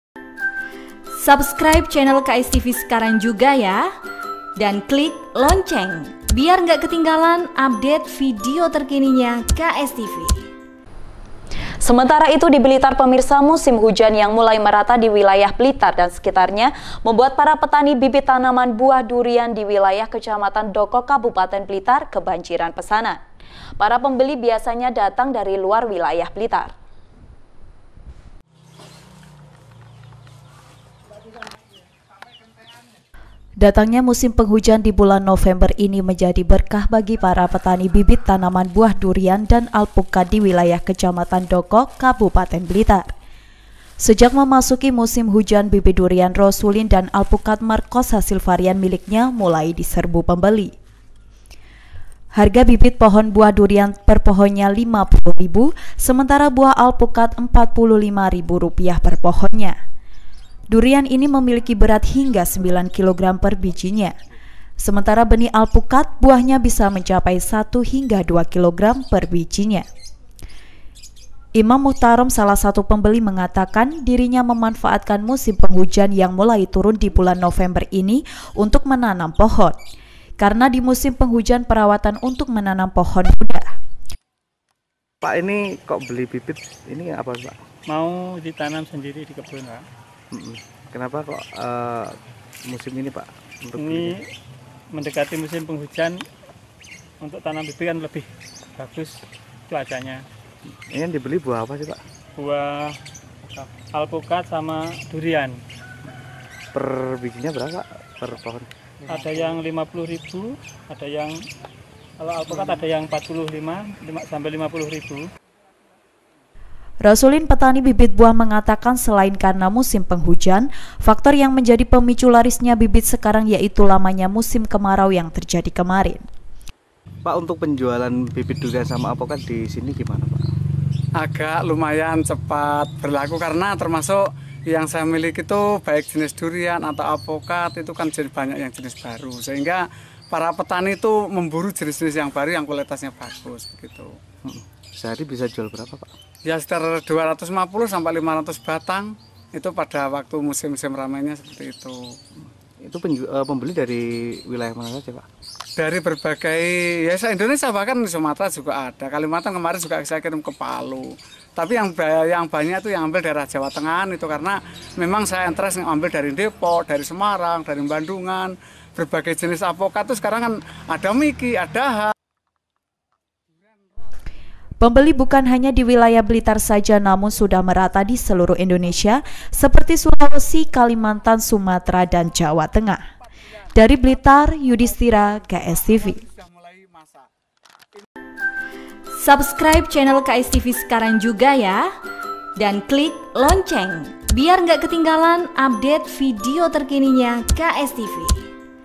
BLITAR - MUSIM HUJAN TIBA , BIBIT DURIAN DAN ALPUKAT DISERBU PEMBELI
Musim hujan yang mulai merata di wilayah blitar dan sekitarnya, membuat para petani bibit tanaman buah durian, di wilayah kecamatan Doko kabupaten Blitar kebanjiran pesanan. Para pembeli biasanya datang dari Luar Wilayah Blitar.